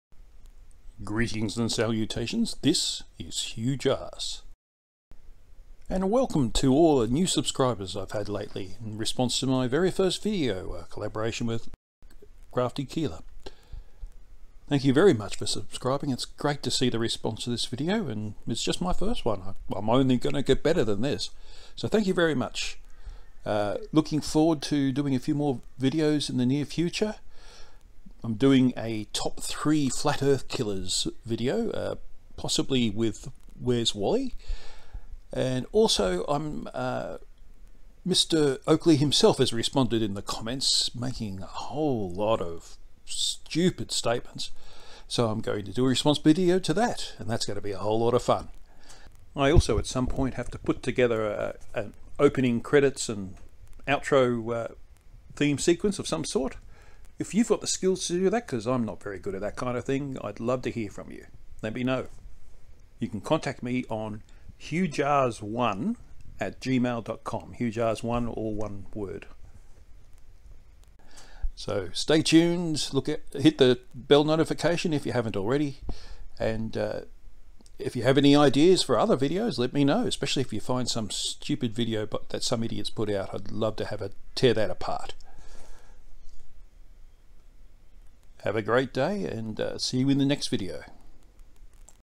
Welcome to new subscribers!
It's April 2020, and my first video (a collaboration with Krafty Keelah) has brought me a bunch of new subscribers. So I'd like to welcome you all properly.